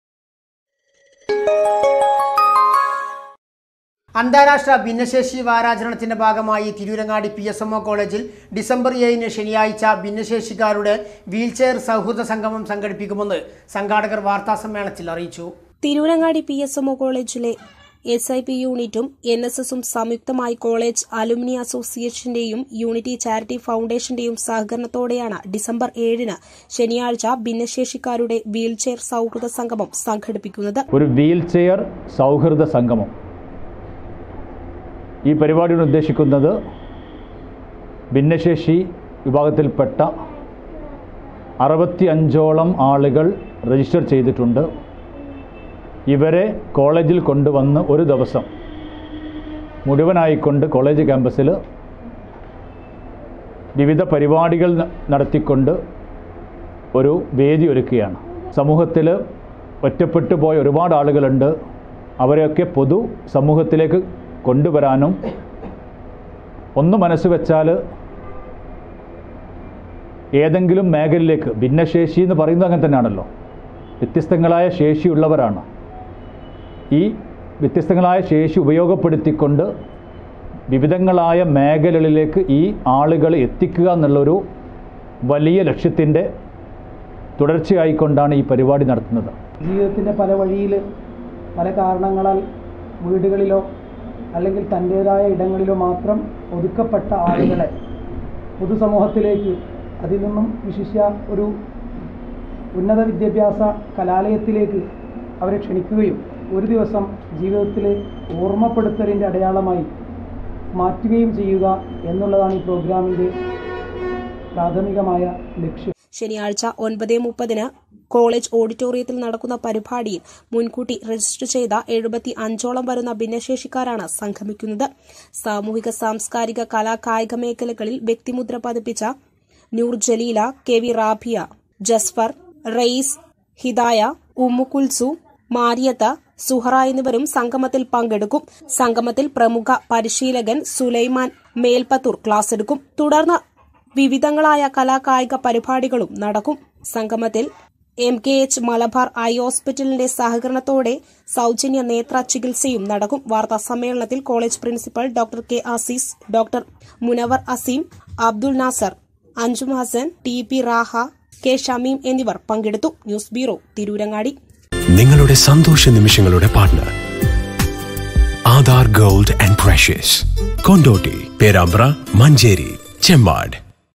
ഭിന്നശേഷിക്കാരുടെ വീല്‍ചെയര്‍ സൗഹൃദ സംഗമം ഏഴിന്
തിരൂരങ്ങാടിപി.എസ്.എം.ഒ കോളേജിലെ എസ്.ഐ.പി.യൂണിറ്റും എന്‍.എസ്.എസ്.ഉം സംയുക്തമായി
കോളേജ് അലുംനി അസോസിയേഷന്റെയും യൂണിറ്റി ചാരിറ്റി ഫൗണ്ടേഷന്റെയും സഹകരണത്തോടെയാണ് ഡിസംബര്‍ ഏഴിന് ശനിയാഴ്ച ഭിന്നശേഷിക്കാരുടെ വീല്‍ചെയര്‍ സൗഹൃദ സംഗമം സംഘടിപ്പിക്കുന്നത്.ശനിയാഴ്ച 9.30 ന് കോളേജ് ഓഡിറ്റോറിയത്തില്‍ നടക്കുന്ന പരിപാടിയില്‍ മുന്‍കൂട്ടി രജിസ്റ്റര്‍ ചെയ്ത 75 ഓളം വരുന്ന ഭിന്നശേഷിക്കാരാണ് സംഗമിക്കുന്നത്.